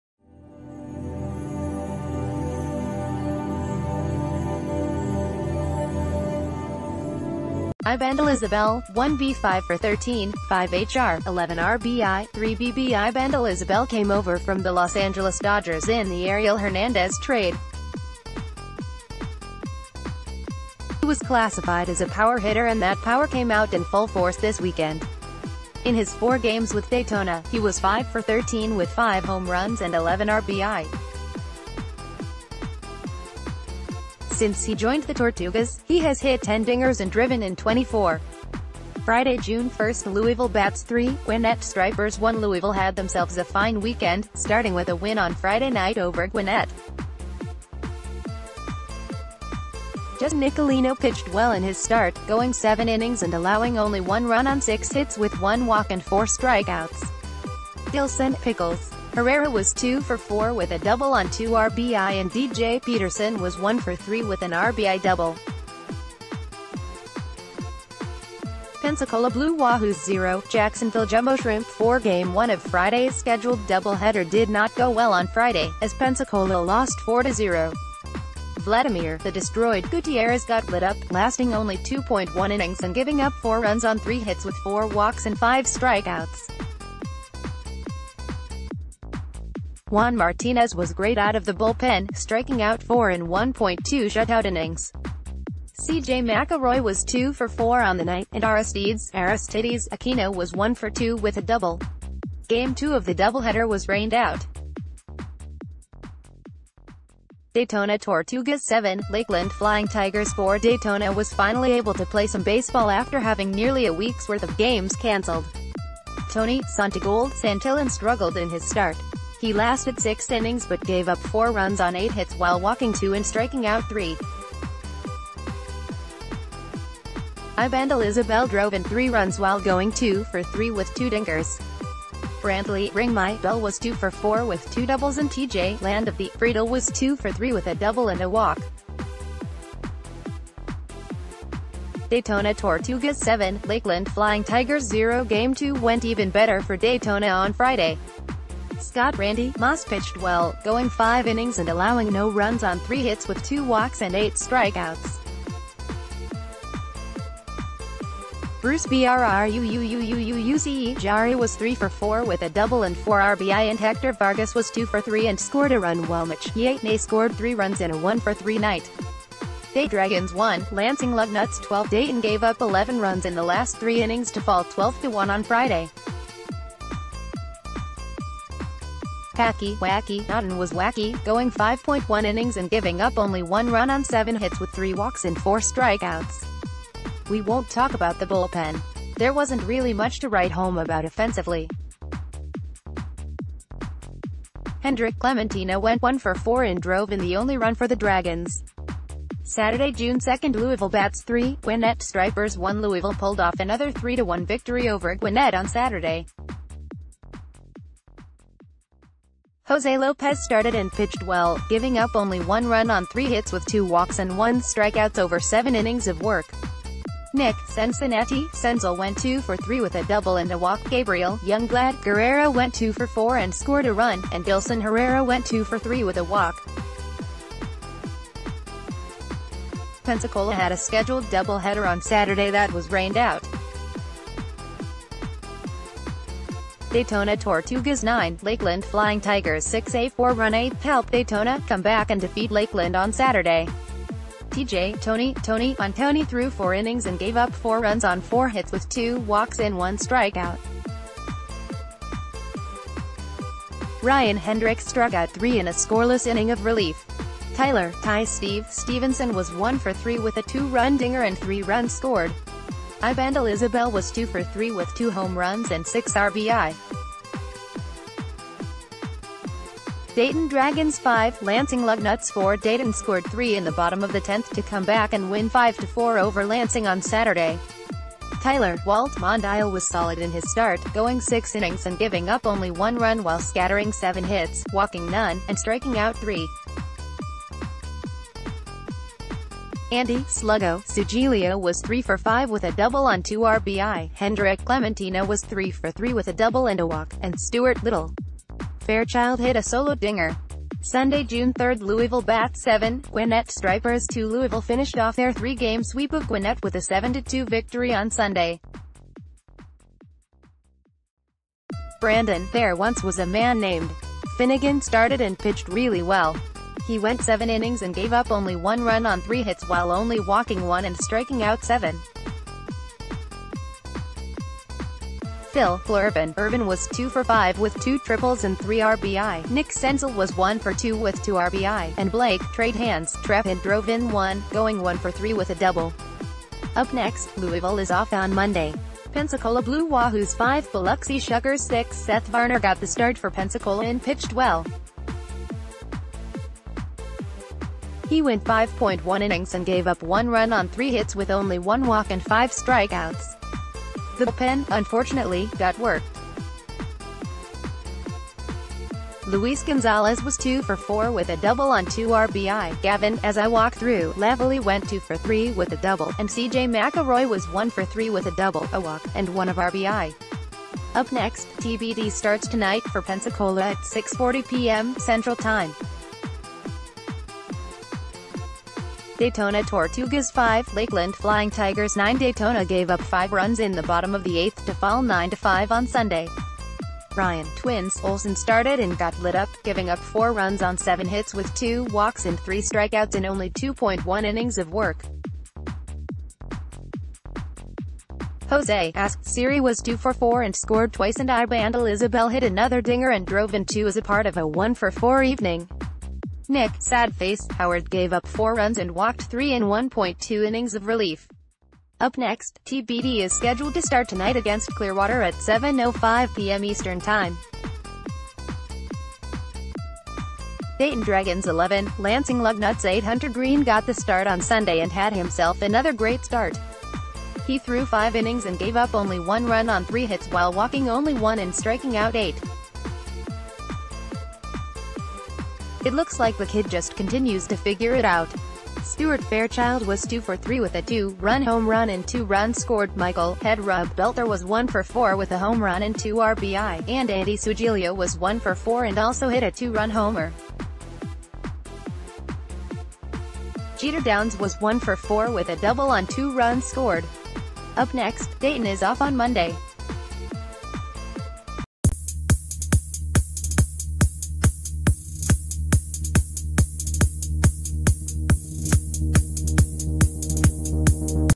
Breaking News | Farmers Only Weekend Harvest: Ibandel Isabel can’t stop hitting dingers
Ibandel Isabel, 1B - 5 for 13, 5 HR, 11 RBI, 3 BB Ibandel Isabel came over from the Los Angeles Dodgers in the Ariel Hernandez trade. He was classified as a power hitter and that power came out in full force this weekend. In his 4 games with Daytona, he was 5 for 13 with 5 home runs and 11 RBI. Since he joined the Tortugas, he has hit 10 dingers and driven in 24. Friday, June 1 Louisville Bats 3, Gwinnett Stripers 1 Louisville had themselves a fine weekend, starting with a win on Friday night ov...